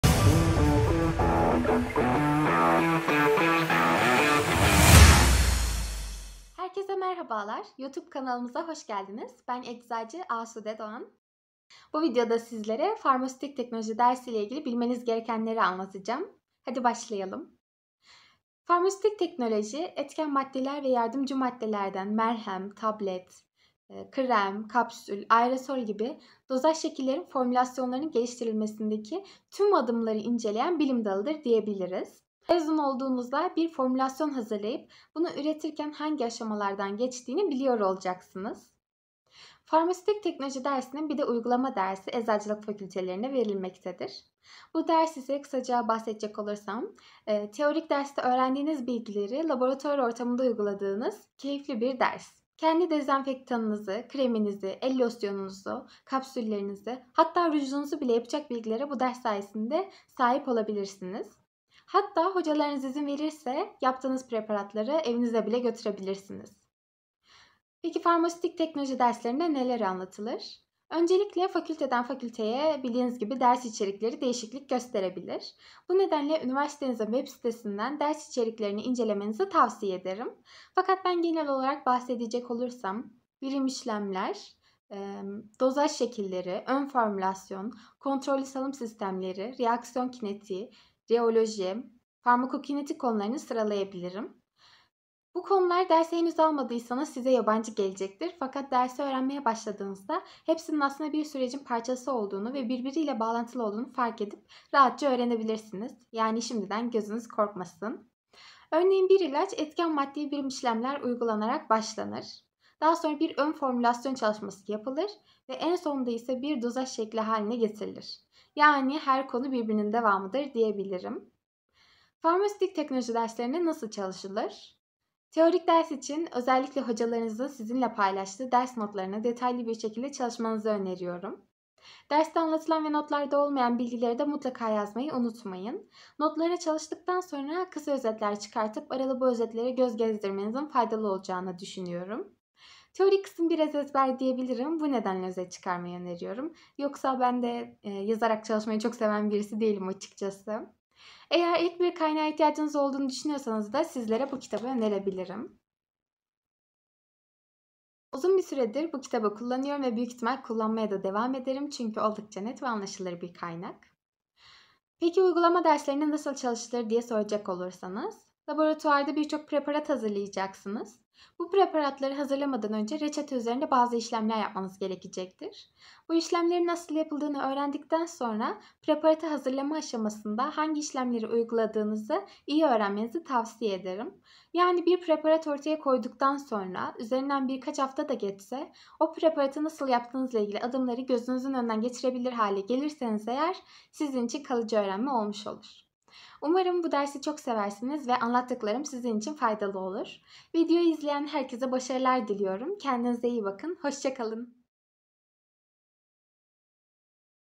ECZ101 : Farmasötik Teknoloji Dersi 🥼💊
Merhabalar! Ecz. Asude Doğan'ın Farmasötik Teknoloji dersi hakkında bilgilendirme yaptığı videoya hoş geldiniz. Videomuzda Farmasötik Teknoloji temellerini, içeriğini ve başarılı bir şekilde nasıl çalışılması gerektiğini ele alacağız.

Farmasötik Teknoloji Nedir?
Farmasötik Teknoloji Dersinin İçeriği?
Farmasötik Teknoloji Nasıl Çalışılmalıdır?
Kaynak Önerisi